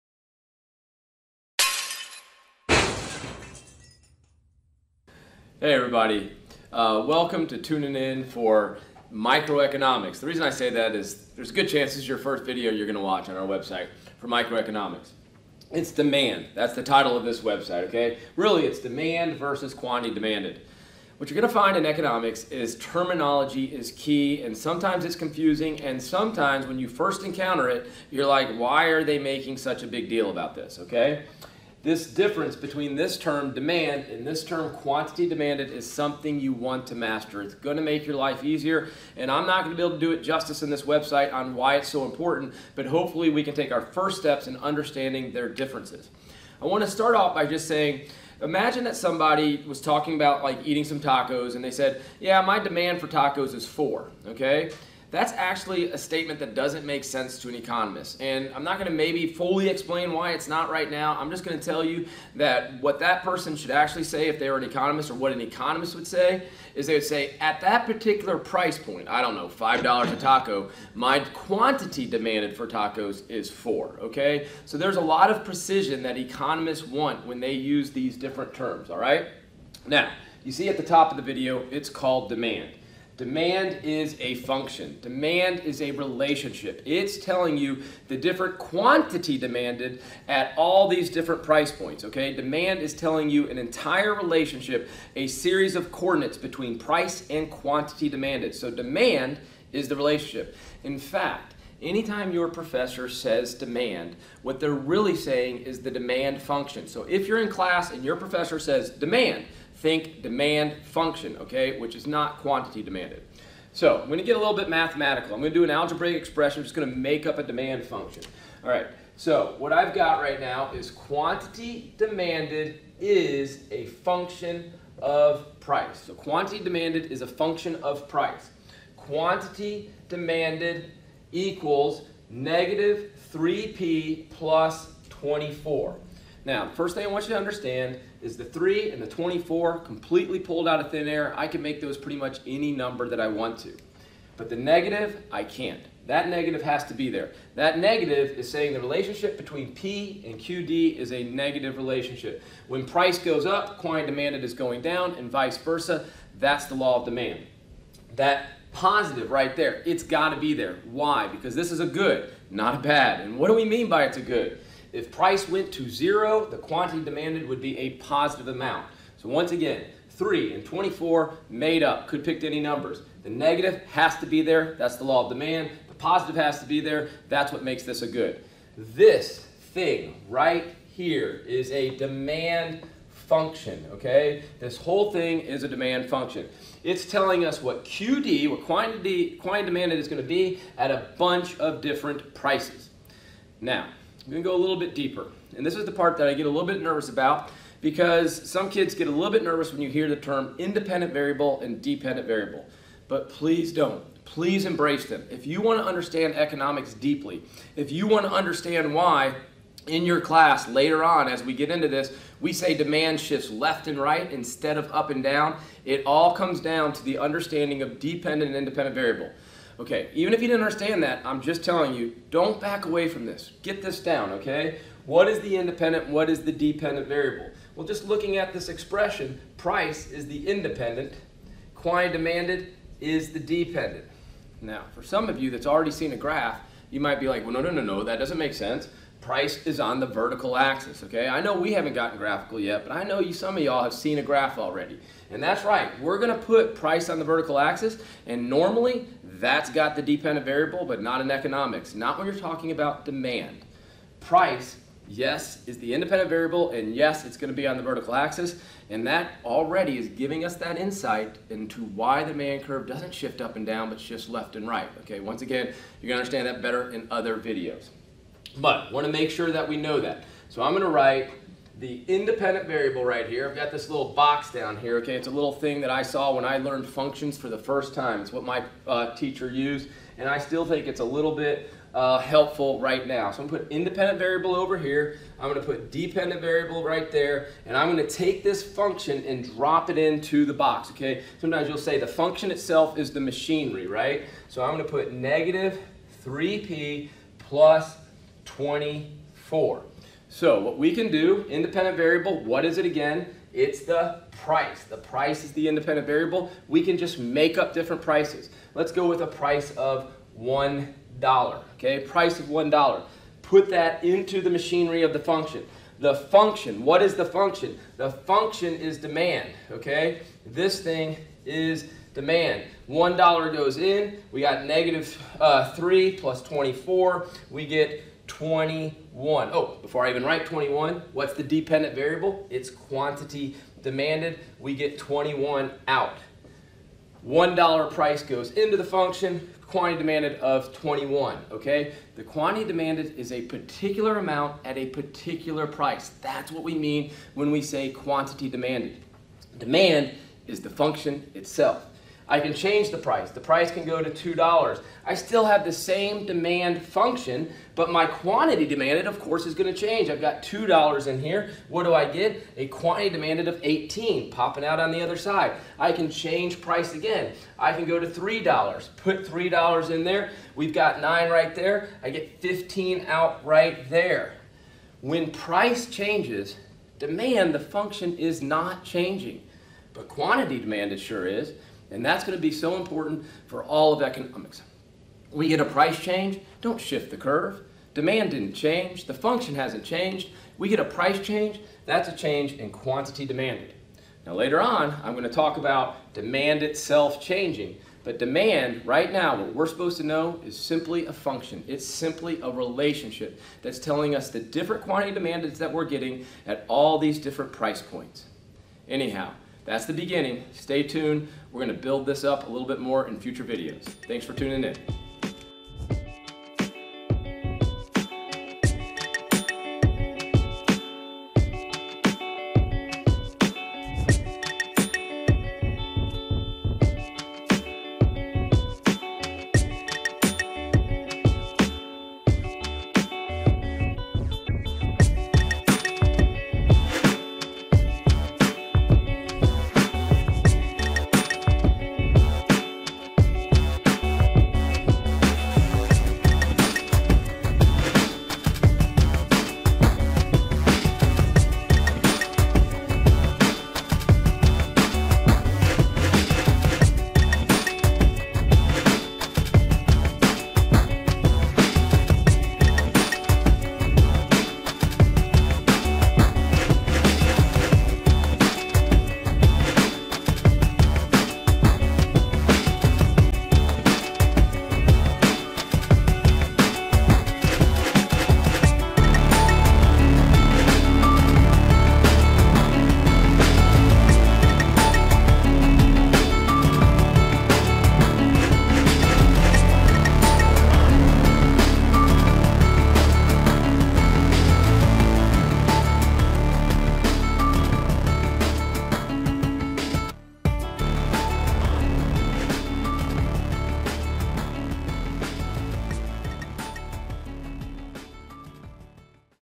The Difference Between Demand and the Quantity Demanded
This video is about the difference between demand and the quantity demanded.  Demand is a function (a relationship) which related price to the quantity demanded.  The quantity demanded is an amount (associated with a specific price). This video is made for 1st year college students or AP/IB Economics students.  It focuses on foundational economic concepts.